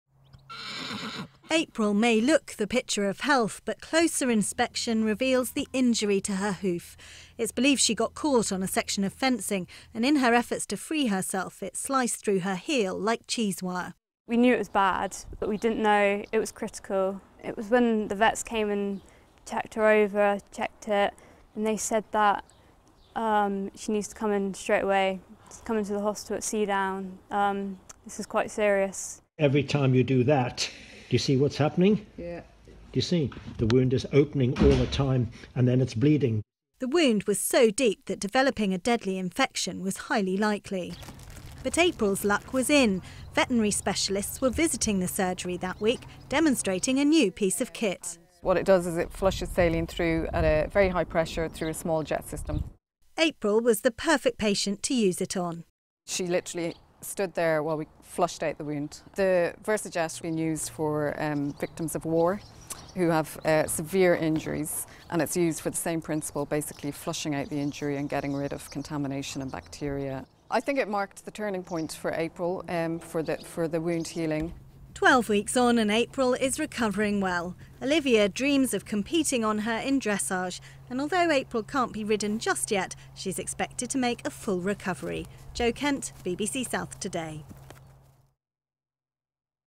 Injured Horse Saved by Iraq Military Medical Invention
Olivia Yendall, 19, feared her Welsh Cob, April, would be put down when its hoof was almost severed by barbed wire.

But while April was in veterinary hospital, visiting equine experts used a specialist machine to treat the wound and the animal is now recovering.

Ms Yendall said it was "a miracle" the horse survived.

Following the incident in May, veterinary wound specialists Prof Derek Knottenbelt and Georgie Hollies, who had been visiting Seadown animal hospital in Hythe, used the specialist saline pump - called a Versajet - which can clean inside the most serious wounds without causing further damage.